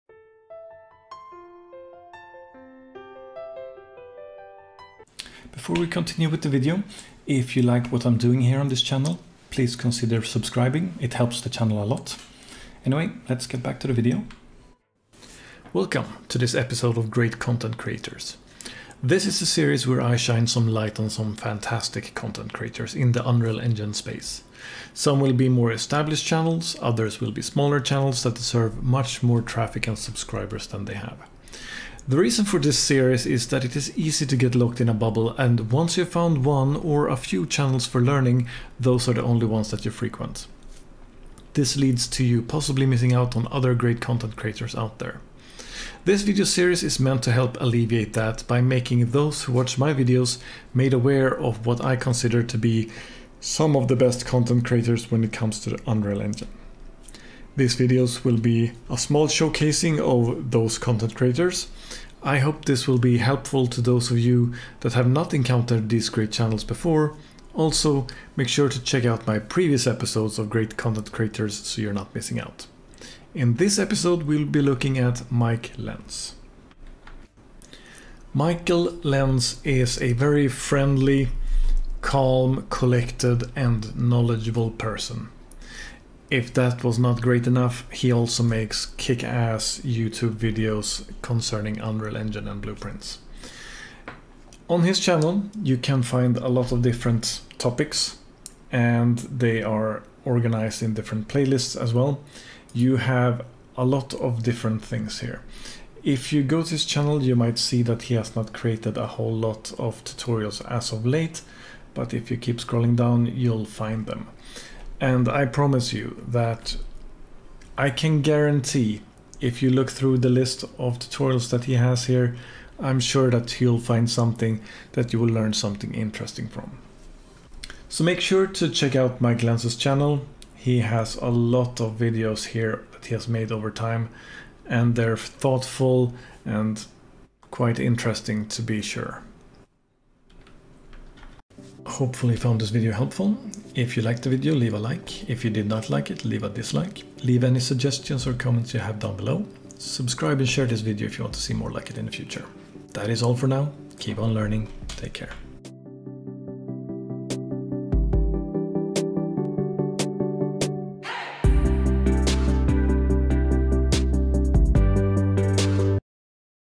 Great content creators #6 - Mike Lenz - Unreal Engine
In this series we take a look at some of the greatest content creators out there related to Unreal Engine.
In this episode we take a look at Mike Lenz.